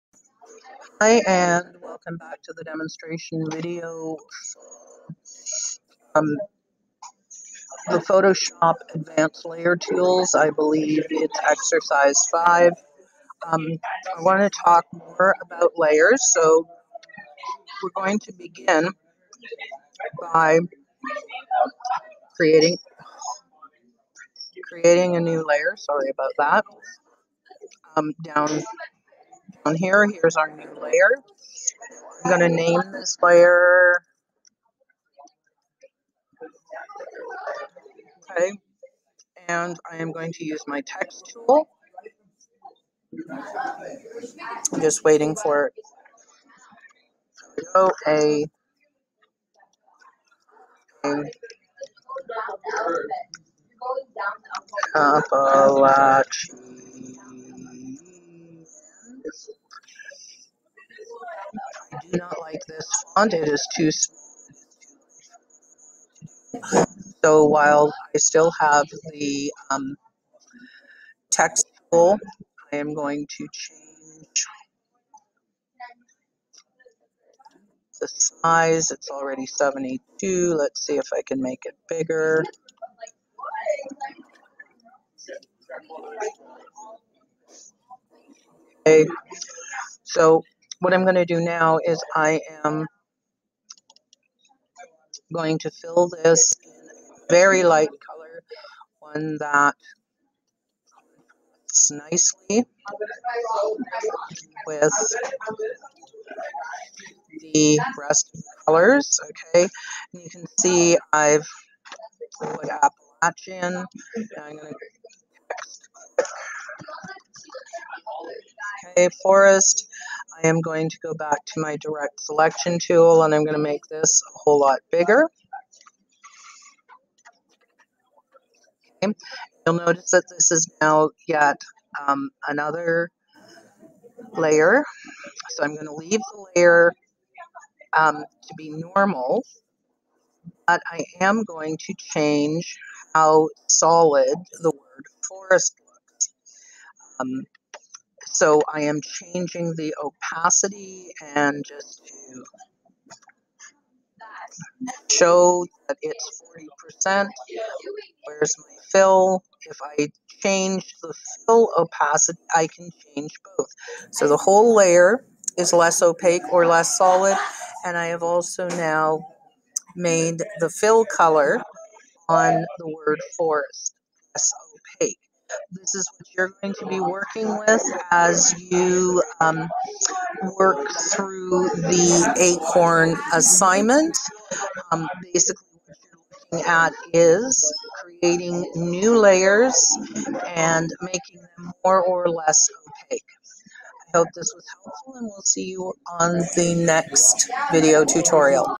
3 5 1 Layer Opacity
Working with multiple layers and creating different levels of opacity (solidity)